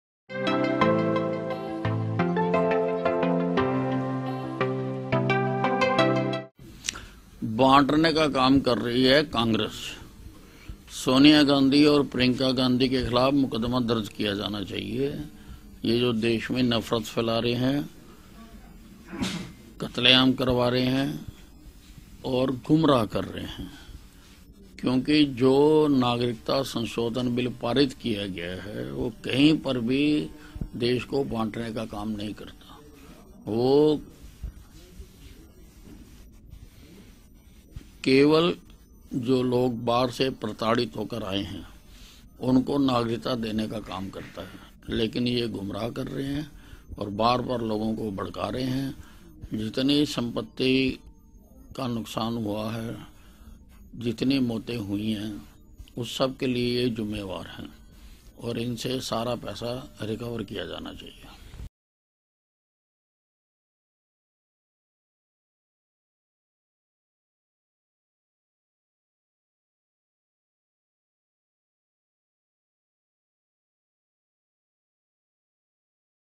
Ambala: Sonia Gandhi पर दर्ज हो देश में नफरत फैलाने का मुकदमा- Anil Vij
सीएए पर कांग्रेस की राष्ट्रिय अध्यक्ष सोनिया गांधी द्वारा दिए ब्यान पर हरियाणा के गृह मंत्री अनिल विज ने बड़ा ब्यान देते हुए कहा कि  CAA  पर कांग्रेस देश को बांटने का काम कर रही है सोनिया गांधी और प्रियंका गांधी पर देअरियाना के गृह मंत्री श में नफरत फैलाने और कत्लेआम करवाने का मुकदमा दर्ज होना चाहिए बार-बार लोगों को भड़काने  के चलते देश की जितनी भी संपत्ति का हुआ नुकसान उसकी रिकवरी सोनिया गांधी और प्रियंका गांधी वाड्रा से होनी चाहिए